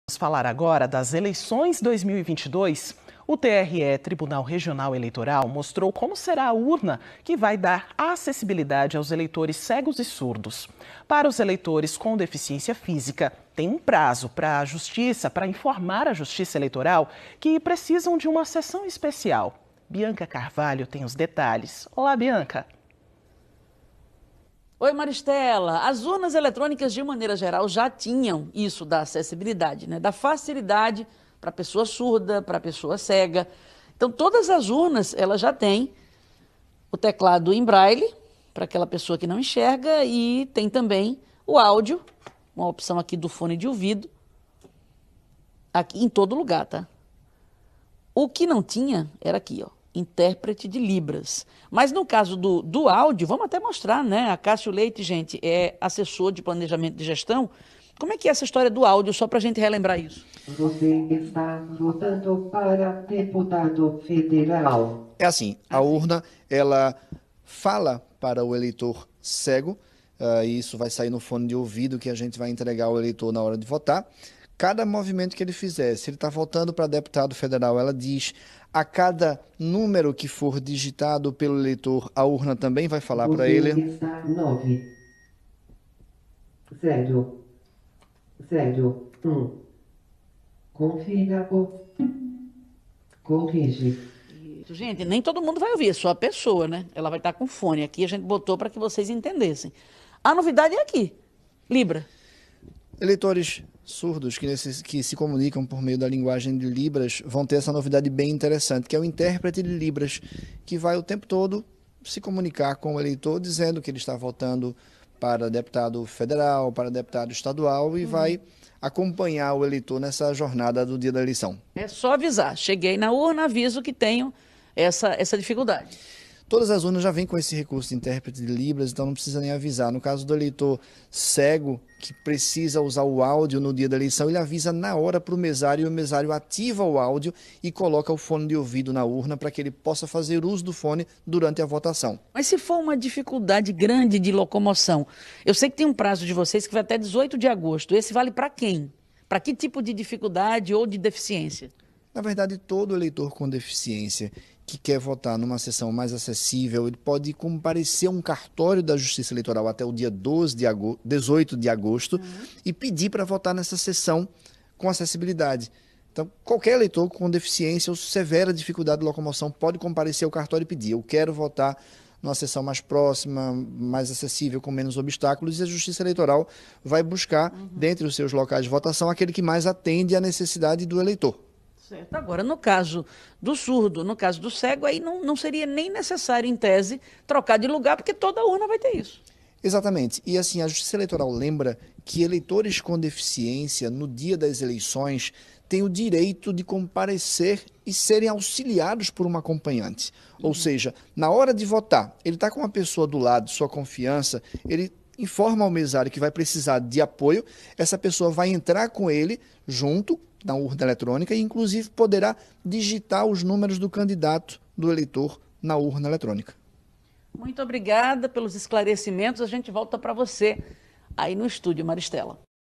TRE-PE apresenta uma nova urna eletrônica para dar acessibilidade para eleitores
Além do além do teclado em braile presente nas votações anteriores, haverá também outros recursos de acessibilidade, como intérprete de libras e audiodescrição com orientação sobre o passo a passo da votação.

Os recursos de acessibilidade estarão presentes em todos os modelos de urna eletrônica para esta Eleição.

✅Confira as informações na entrevista de Acácio Leite, Assessor de Planejamento, ao NE1 de 26 de julho.